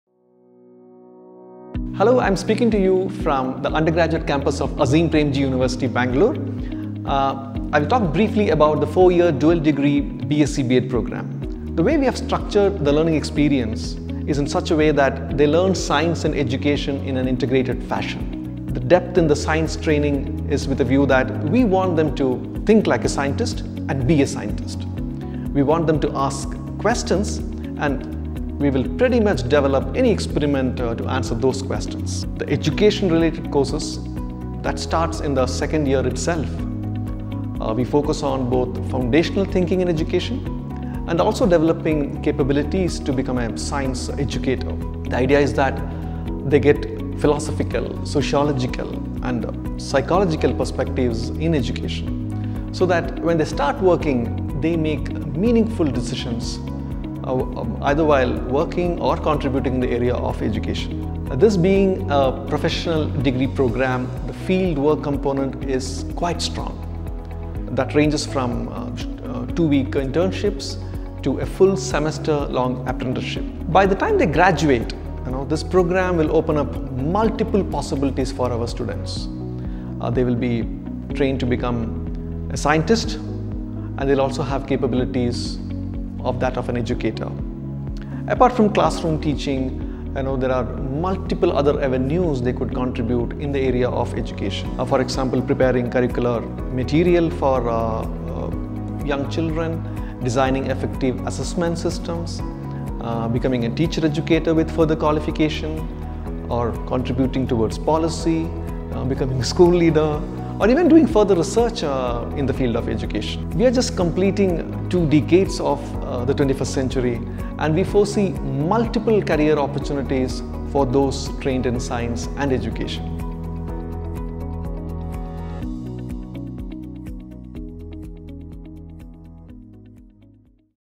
Azim Premji University B.Sc B.Ed Overview | Rajagopal | Travelling Tripod Films
Rajagopal, in charge of admissions at Azim Premji University speaking on the B.Sc B.Ed programme.